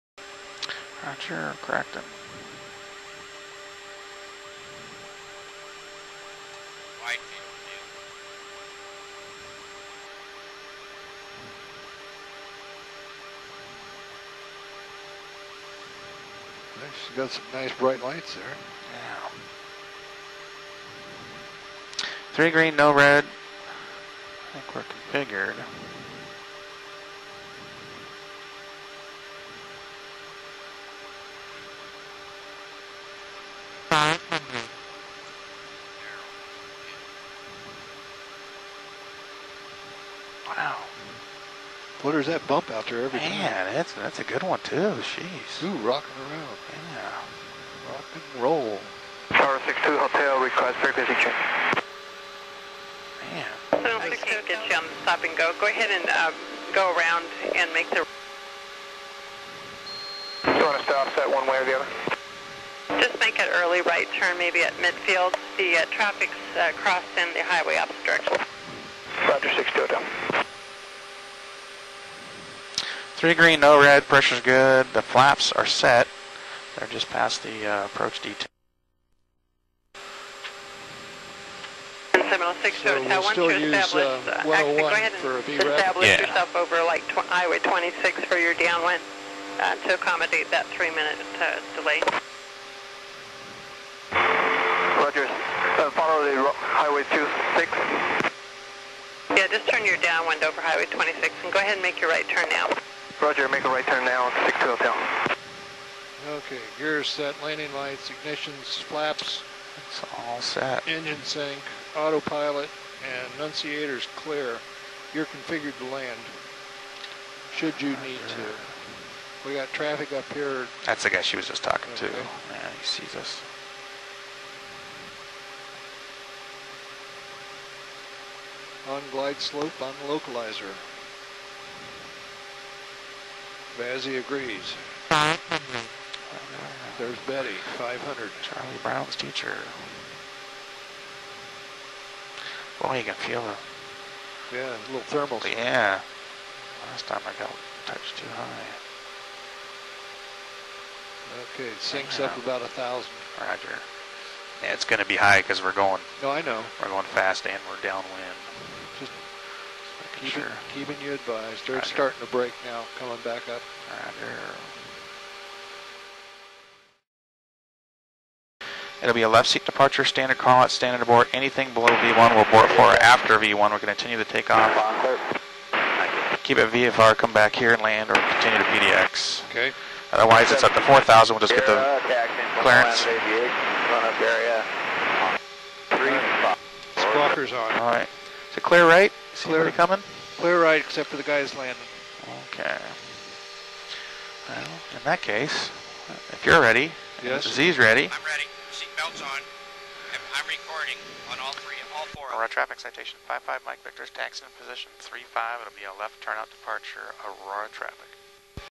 EVS-1500 Flight Test Trials 2007
On approach comparisons of EVS view to a witness view and comparison of the EVS-1500 in narrow field of view versus wide fov